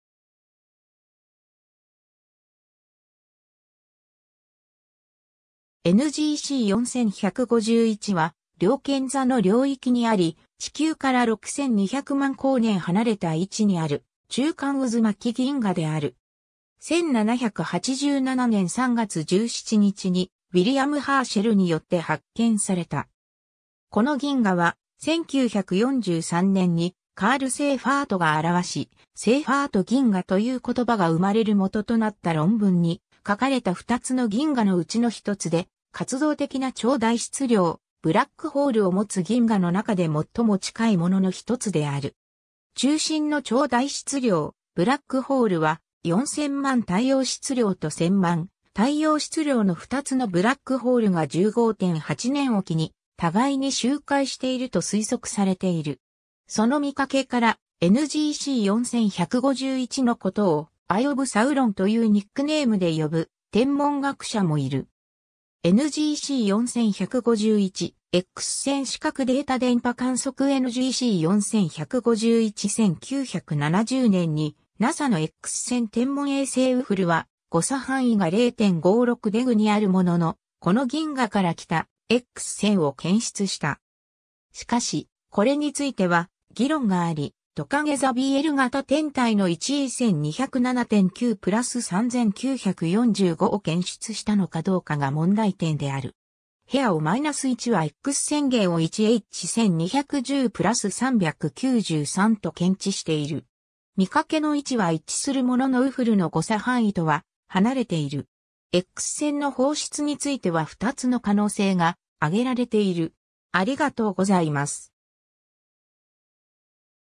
NGC 4151
NGC 4151は、りょうけん座の領域にあり、地球から6200万光年離れた位置にある中間渦巻銀河である。
1787年3月17日にウィリアム・ハーシェルによって発見された。
この銀河は、1943年にカール・セイファートが著し、「セイファート銀河」という言葉が生まれる元となった論文に書かれた2つの銀河のうちの1つで、活動的な超大質量ブラックホールを持つ銀河の中で最も近いものの1つである。
中心の超大質量ブラックホールは、4000万太陽質量と1000万太陽質量の2つのブラックホールが15.8年おきに互いに周回していると推測されている。
 その見かけから、NGC 4151のことを「Eye of Sauron」というニックネームで呼ぶ天文学者もいる。
 NGC 4151. X線(青)視覚データ(HII領域)(黄色)電波観測(赤) NGC 4151(ハッブル宇宙望遠鏡) 1970年にNASAのX線天文衛星ウフルは、誤差範囲が0.56deg2あるものの、この銀河から来たX線を検出した。
しかし、これについては議論があり、とかげ座BL型天体の1E 1207.9 +3945を検出したのかどうかが問題点である。
HEAO-1はX線源を1H 1210+393と検知している。
見かけの位置は一致するもののウフルの誤差範囲とは離れている。
 X線の放出については2つの可能性が挙げられている。